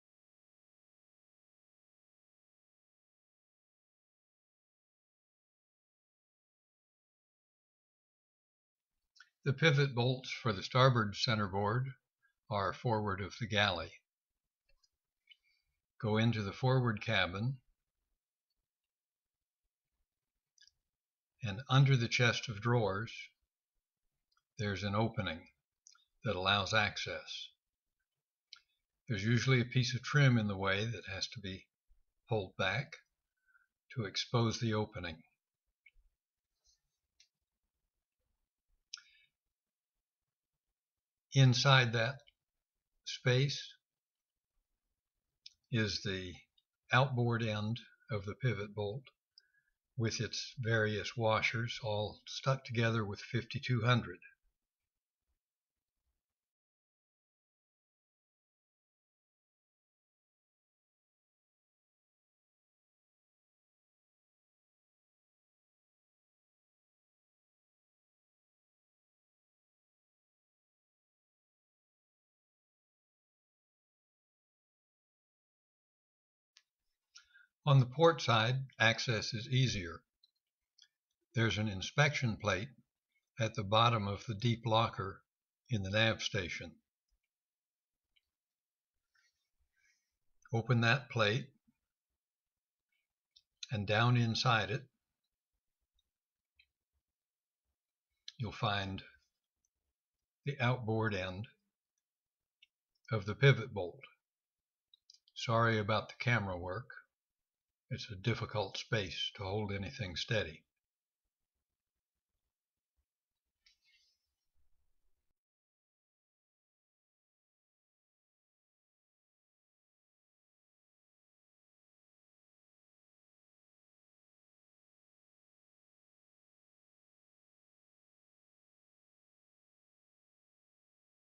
Gemini Centerboard Bolts
This shows you where to get access to the outboard ends of the pivot bolts on the Gemini centerboards.